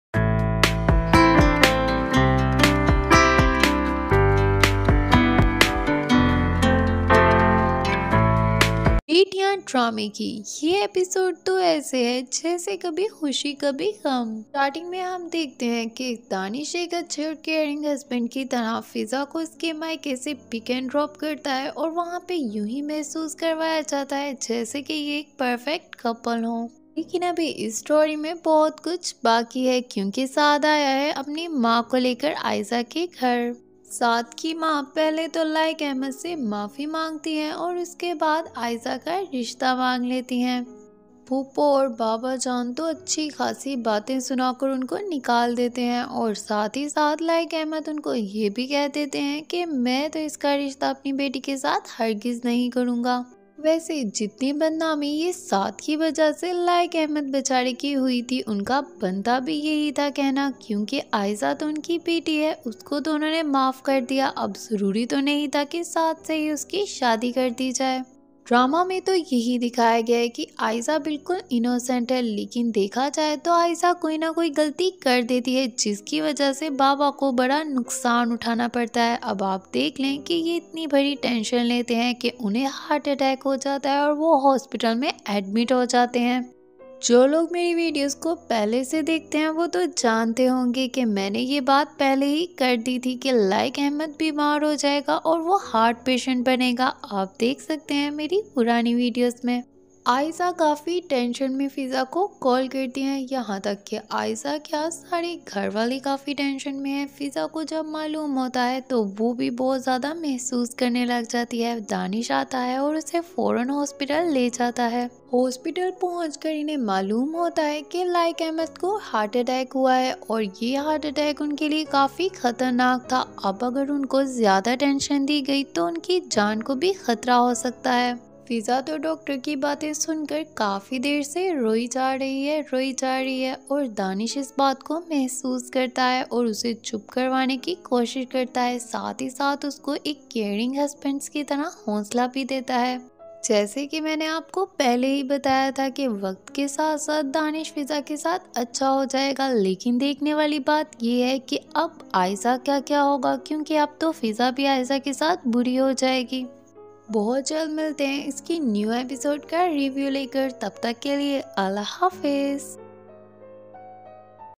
Betiyaan Episode 16- 24th October 2022- Drama Review||ARY digital||Presented by Maria Javed
Betiyaan is a story of an affectionate father, Laiq Ahmad, going through immense difficulties for the love of his daughters. Apart from the difficulties created by his mother and divorced sister, he also has to deal with the dirt society throws at him .

Due to a tittle misunderstanding , his family is falling apart and Laiq Ahmad is desperate to turn things around for his family. What lengths with he go to ?

Cast
Fatima Effendi,
Muhammad Ahmad,
Mah-e-Noor Haider,
Fahad Sheikh
Qudsia Ali,
Tania Hussain,
Emaan Khan,
Beena Masroor,
Javeria Masroor
Shahrayar Zadi
Sabahat Bukhari
Sajjad Pal,
Saad Fareedi,